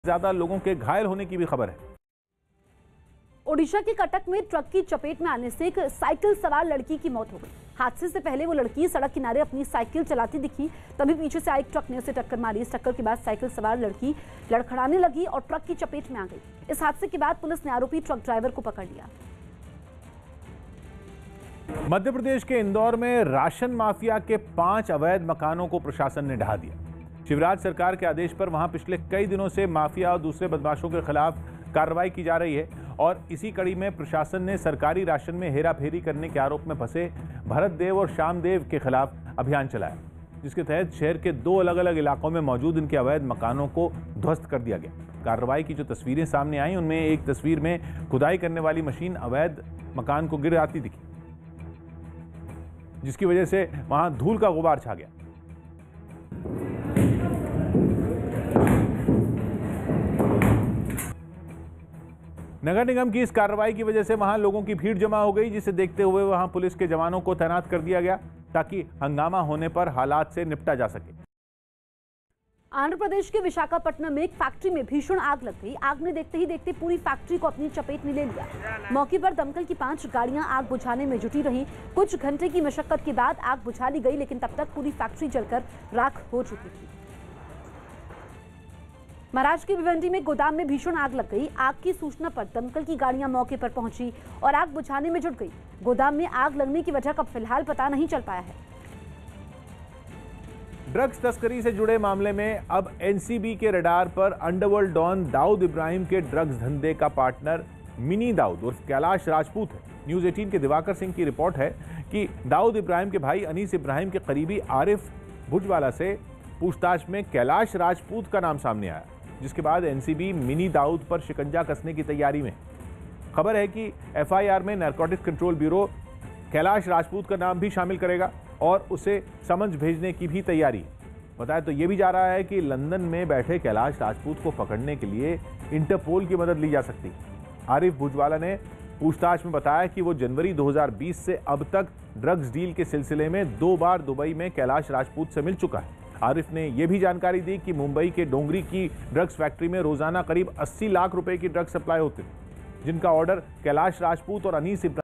Odisha : Katak में ट्रक की चपेट में आने से साइकिल सवार लड़की की मौत, ट्रक चालक को पुलिस ने पकड़ा
Odisha के Katak में ट्रक की चपेट में आने से रोड के साइड मे साइकिल चल रही लड़की को ट्रक ने कुचल दिया.  साइकिल सवार लड़की ट्रक से टकराने के बाद लड़खड़ाई और फिर ट्रक के नीचे आकार उसकी मौत हो गई. बाद में पुलिस ने ट्रक चालक को धरदबोचा.